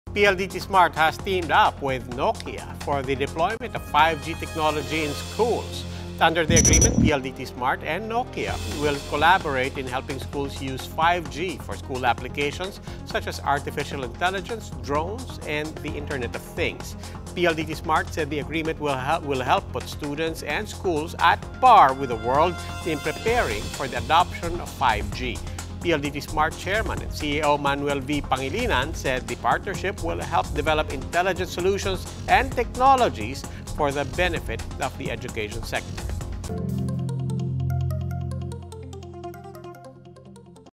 Smart, Nokia team up for 5G in schools
PLDT-Smart has teamed up with Nokia for the deployment of 5G technology in schools.